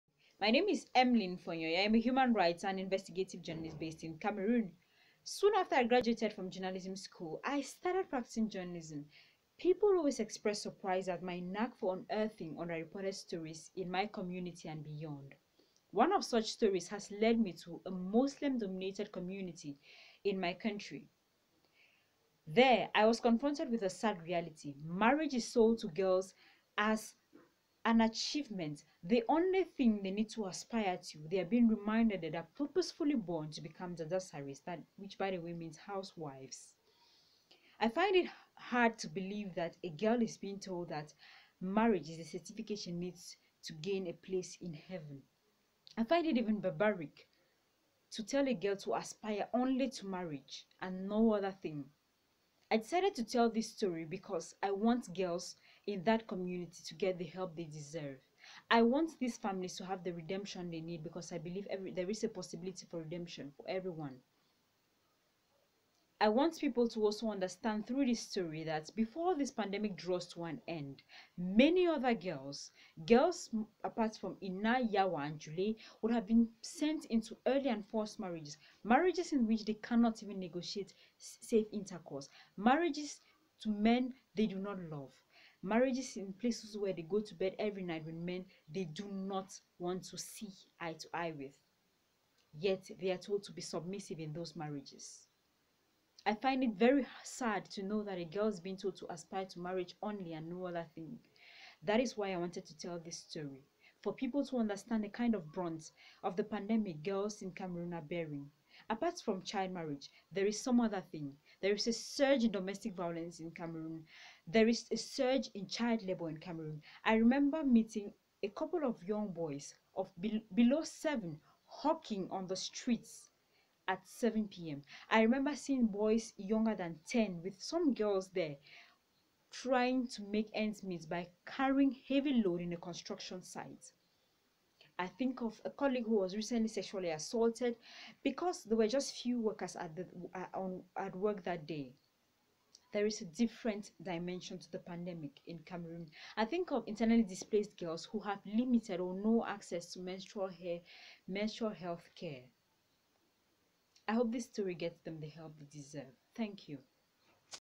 Reporter’s View: Emeline Fonyuy on child marriage in Cameroon
Emeline Fonyuy is a freelance journalist based in Cameroon covering women’s issues. Fonyuy is also a communications officer for Cameroonian gender rights NGO Leap Girl Africa. This article was written in her personal capacity as a journalist.